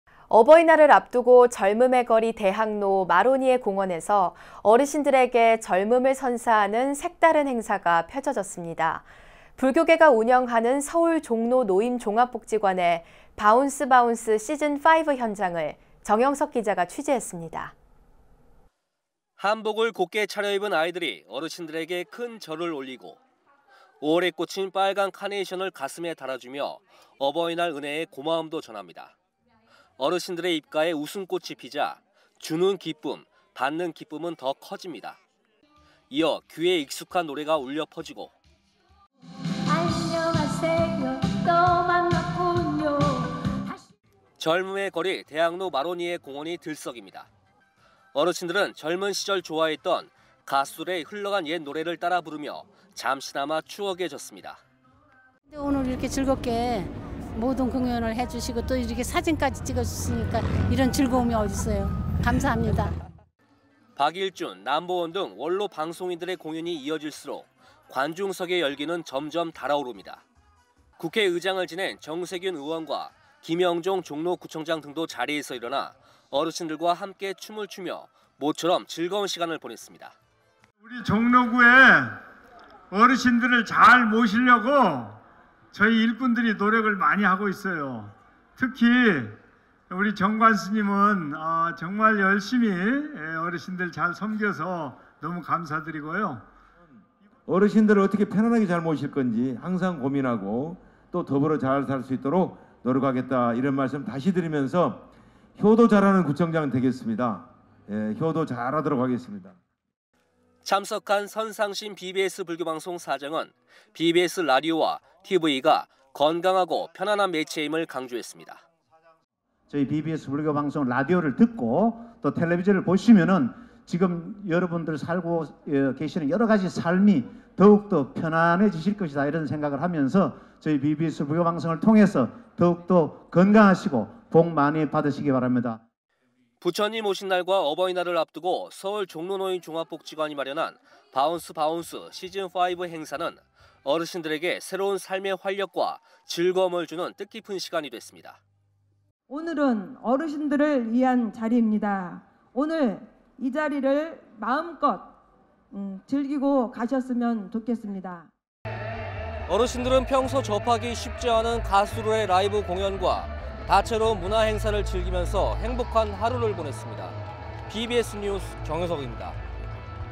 [BBS뉴스] 종로노인종합복지관 “어버이날, 젊음을 선물로”
부처님오신날과 어버이날을 앞두고 젊음의 거리 대학로 마로니에 공원에서 어르신들에게 젊음을 선사하는 색다른 행사가 펼쳐졌습니다. 불교계가 운영하는 서울 종로노인종합복지관의  ‘바운스바운스 시즌5’ 현장을 정영석 기자가 취재했습니다.

▶ BBS를 다양하게 만나보세요!